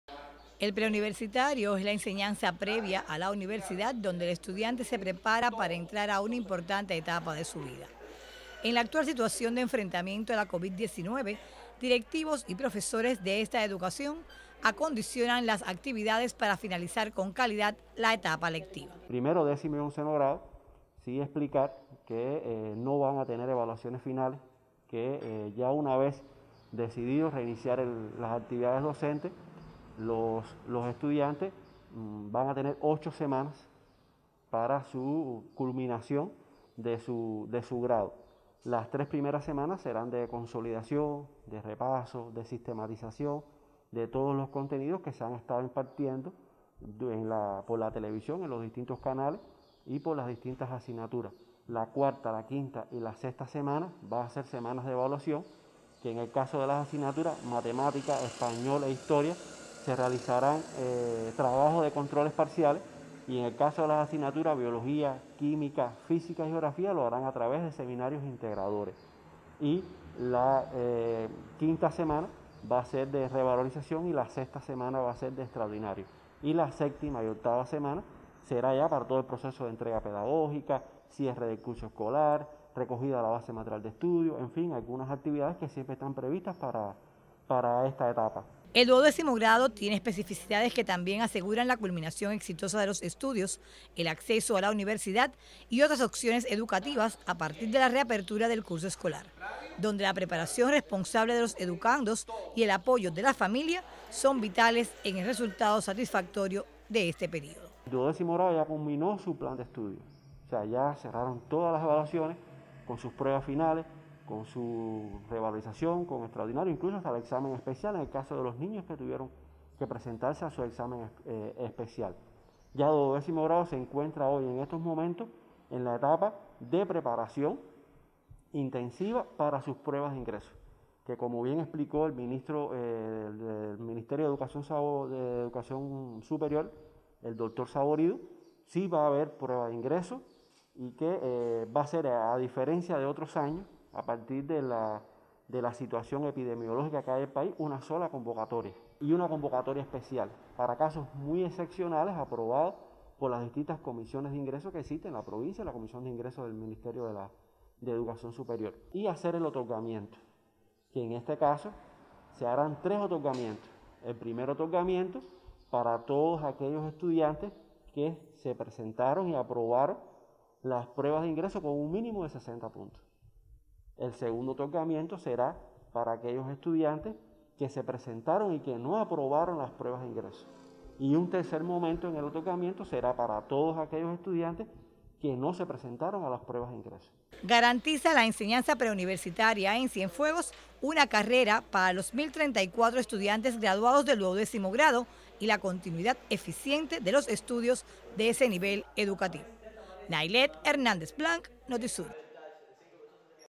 Se prepara la enseñanza preuniversitaria en Cienfuegos para finalizar el curso escolar 2019-2020
A partir de las características de la enseñanza preuniversitaria, la culminación de estudios del presente período lectivo se ha diseñado por el país dos momentos importantes. En Cienfuegos los directivos y docentes de este nivel educativo realizan acciones para garantizar el cumplimiento de los objetivos docentes para cada grado.